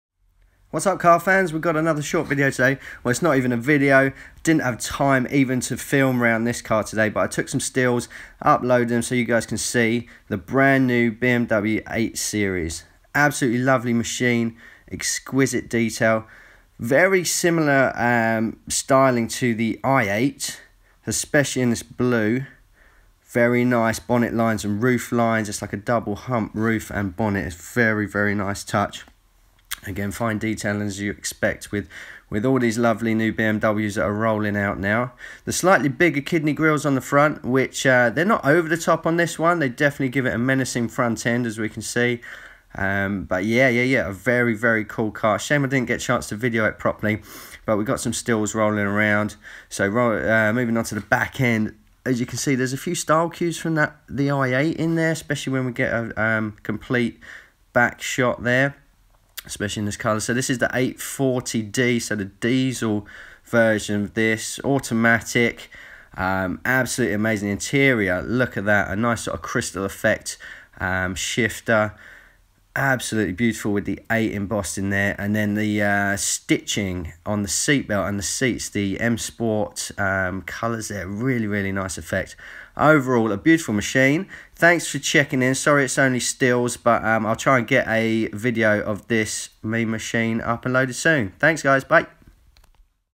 BMW 8 series walk round stills. BMW M sport 840d 2019
A short walk round video using still photos of the brand new BMW 8 series. This particular model is the 840d M Sport.

Sorry didnt have time to do a proper video but wanted to post something about it as its an amazing machine.